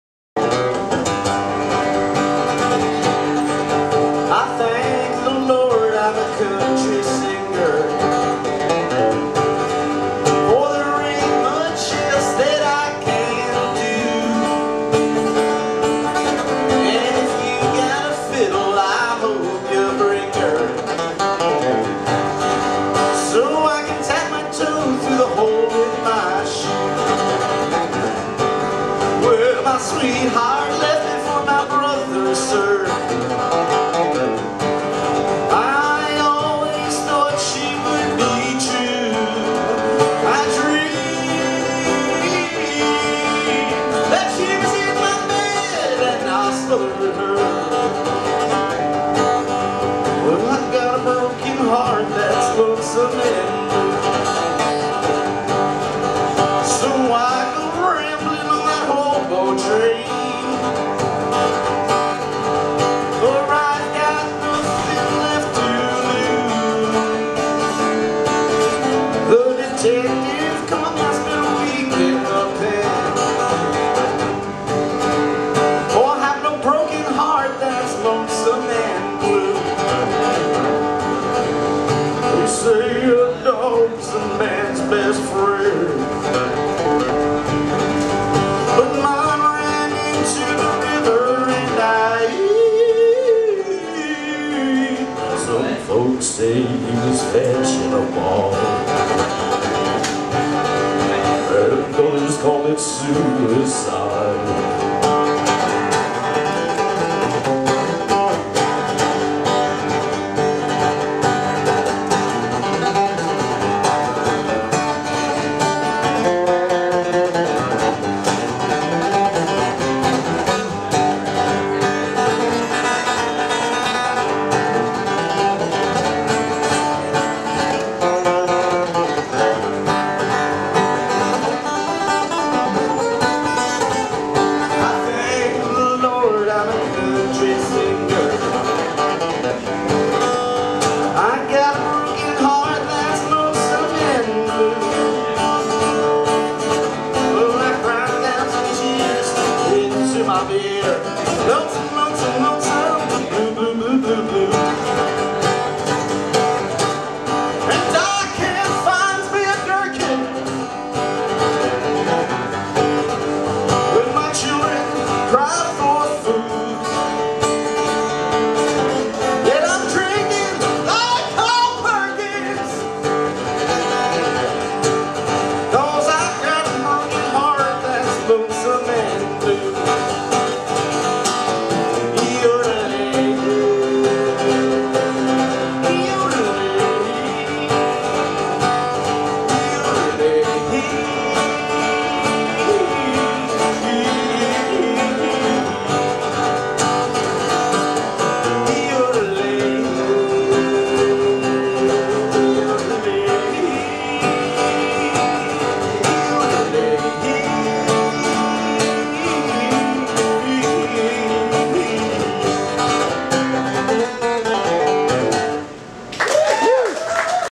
John Kessel - Country Singer
John played the "On the Border Songwriter Series" in the East Village of NYC twice in the summer of 2008. It was an honor to present him and glad to be able to preserve this piece of his solo performance.